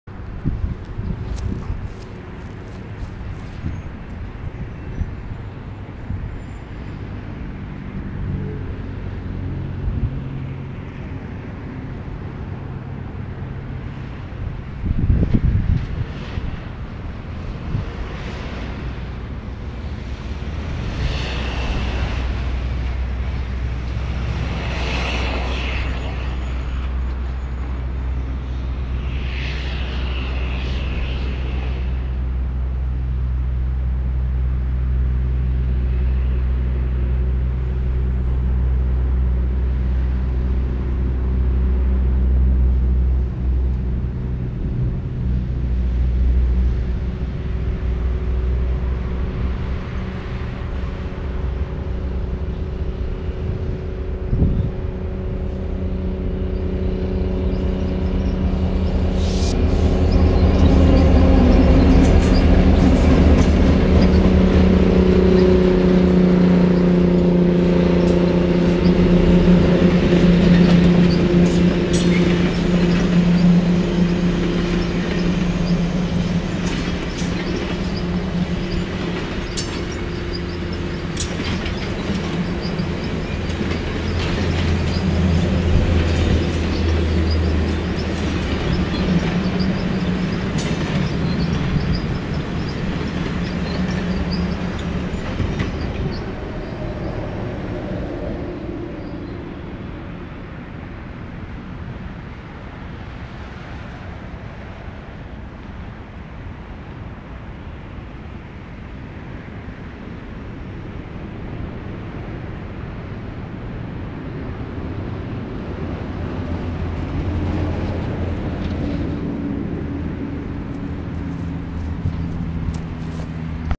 CP 7019, 1401 & 1900 on Business Train 40B Eastbound - 9/23/21
Taken September 23rd, 2021. Skip to 0:50 for the train, lol.

Here's another angle of the 40B that passed through heading eastbound towards the Ex-CMQ tracks. SD70ACu 7019, a maroon-gray 'block letter' HU lead the train along with oddly set up F9A-F9B's 1401 and 1900.

Main video is on the main channel, TWR of course!

 :0